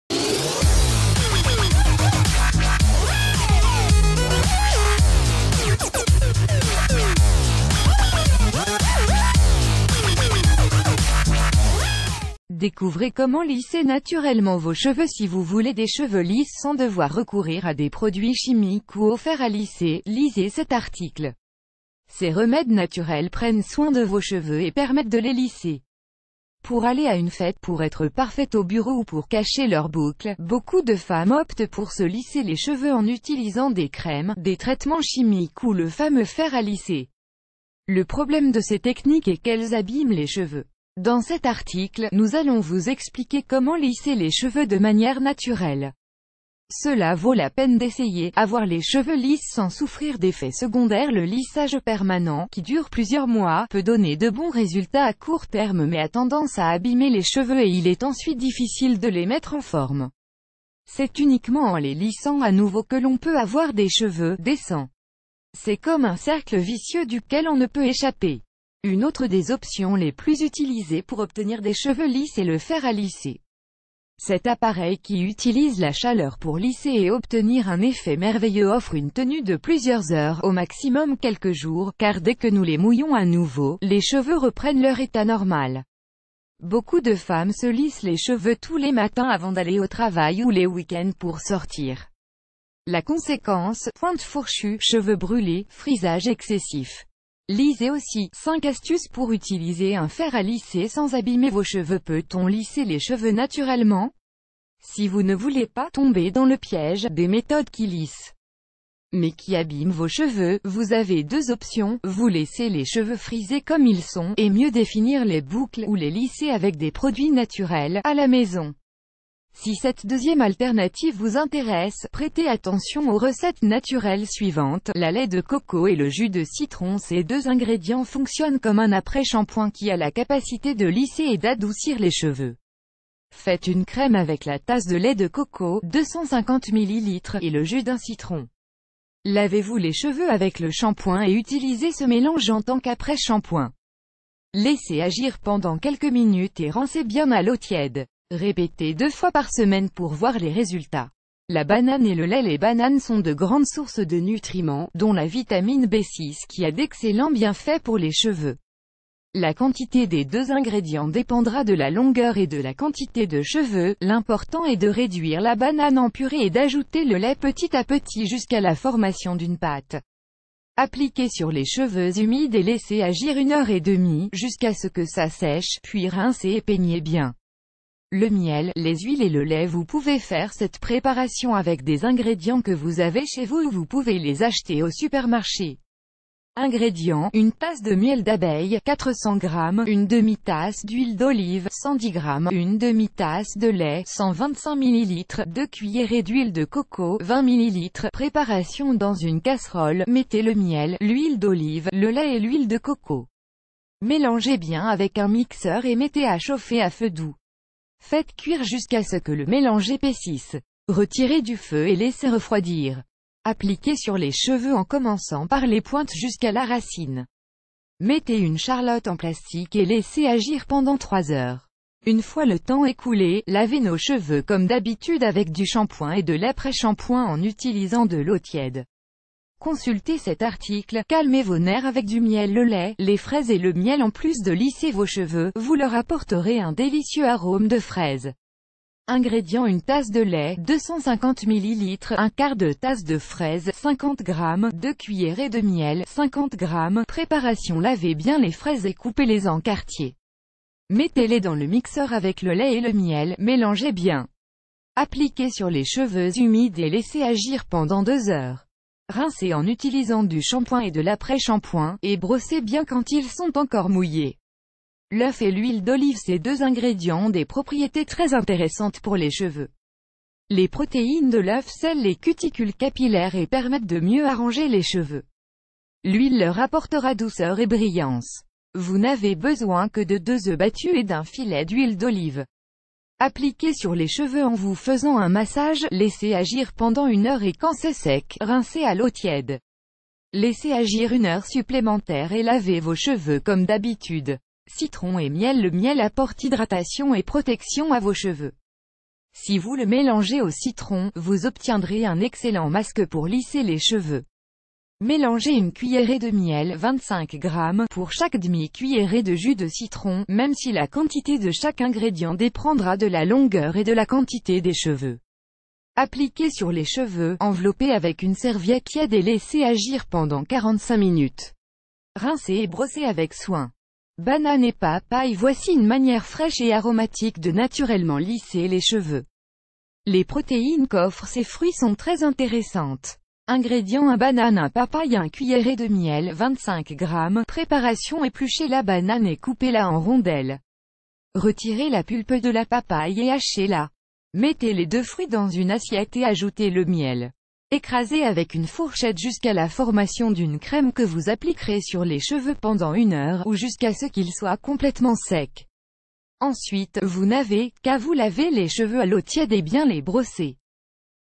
Découvrez comment lisser naturellement vos cheveux
Découvrez comment lisser naturellement vos cheveux Si vous voulez des cheveux lisses sans devoir recourir à des produits chimiques ou au fer à lisser, lisez cet article. Ces remèdes naturels prennent soin de vos cheveux et permettent de les lisser. Pour aller à une fête, pour être parfaite au bureau...